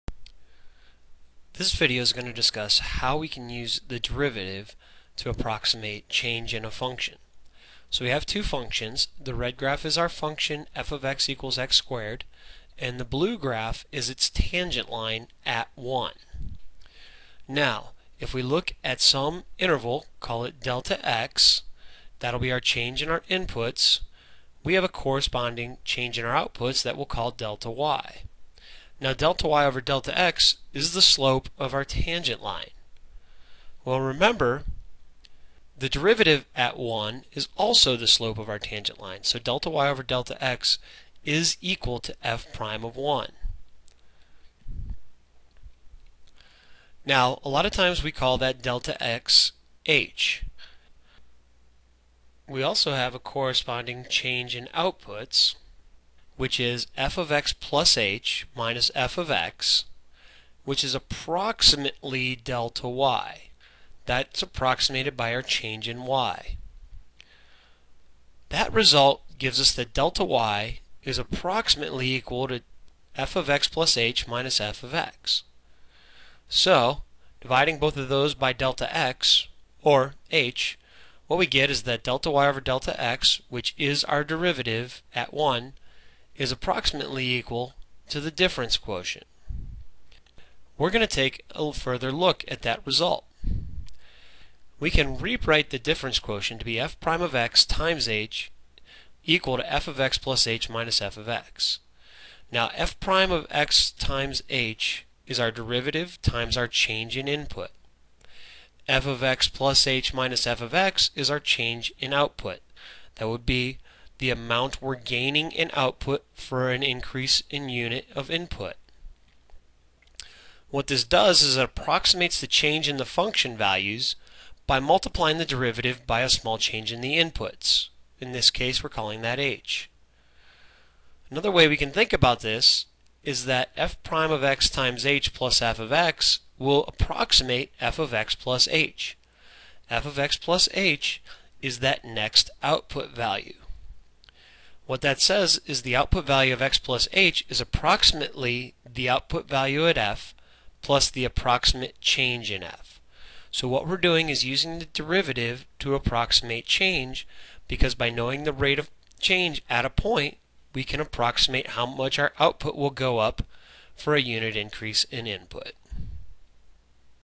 Differentials in Calculus
This video discusses how to use the derivative to approximate change.